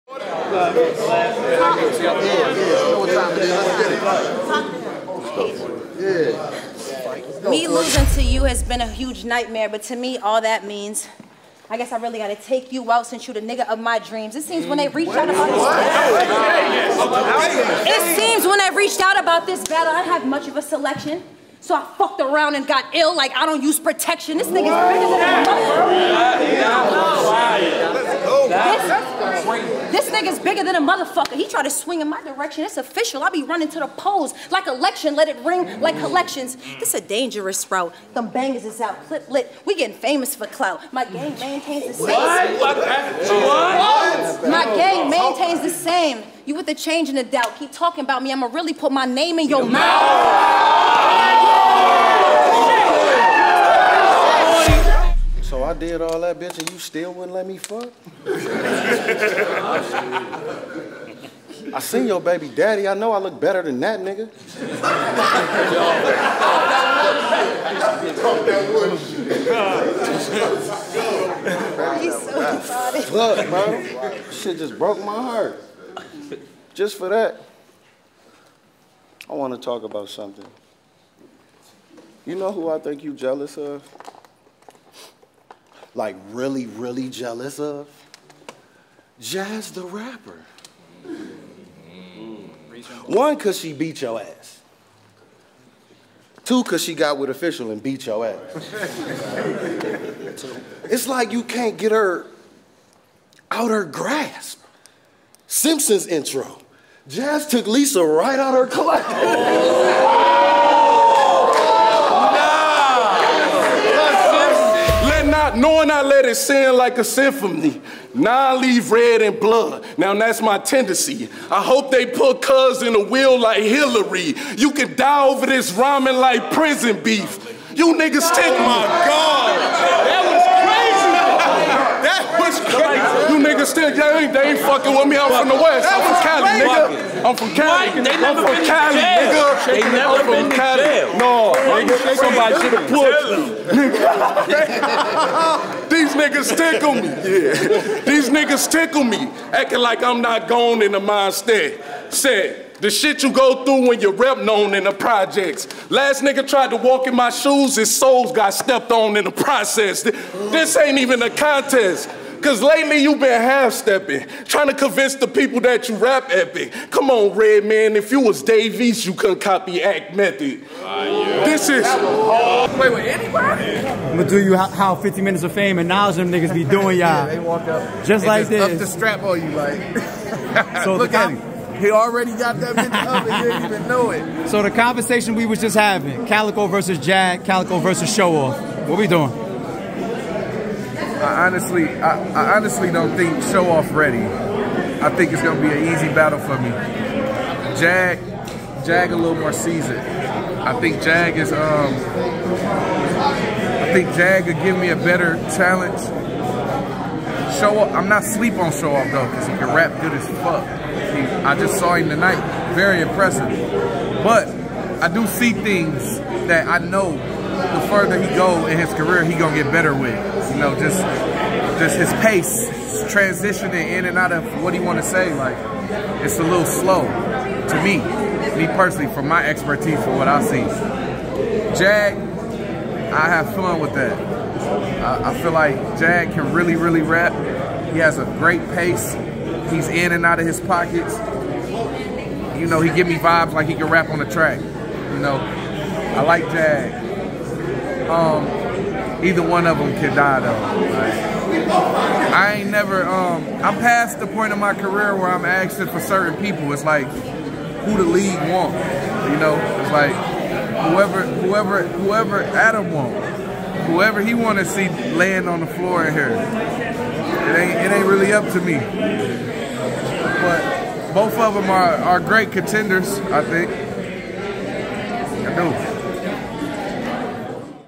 CALICOE ADDRESSES NEXT OPPS ON RBE
Calicoe addresses his next opps on RBE.

Ill Will vs 40 Barrs
O Red vs Aktive
Showoff vs Jag
Ooops vs Real Deal
E Ness vs Gwitty
Bad Newz vs KD
Riddick vs Infrared The General

Follow the Team:
InstaGram - @itsarp @rarebreed_ent @lawrence_rbe @wingedtruth_thebreakdown
Twitter - @itsarp @rare_breed_ent @lawrence_rbe @MzLeta_Truth